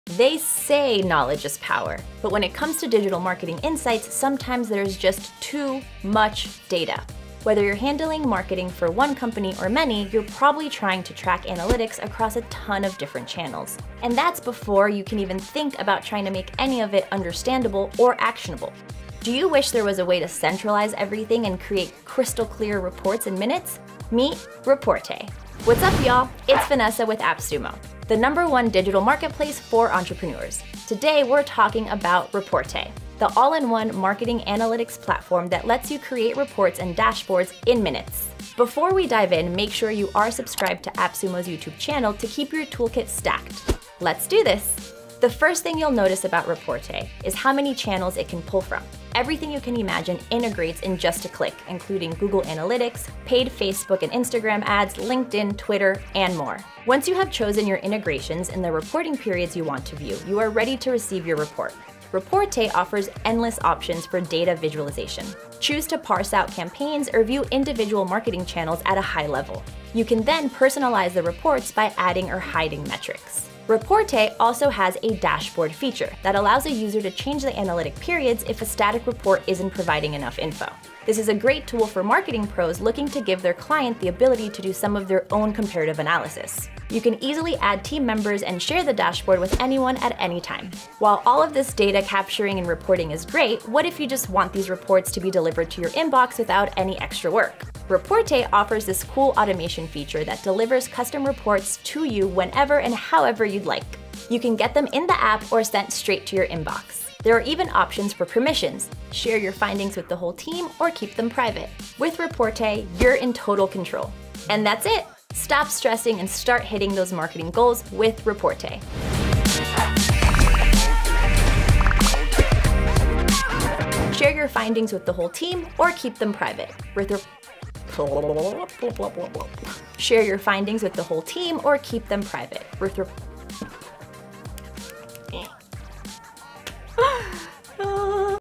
Create Digital Marketing Reports in Seconds with Reportei
Reportei empowers your digital marketing strategy by letting you generate and analyze reports and dashboards across all your channels in seconds.

If you’ve ever been in an overactive group chat (or my family’s Thanksgiving dinner), you know what it’s like dealing with information overload. (Yes, dad, you showed me the rash already.)

But when it comes to digital marketing, you need all that TMI to make sure you’re leveraging content as effectively as possible.

If only you didn’t have to waste time manually collecting data, so you could focus on strategizing with your clients.

Enter Reportei.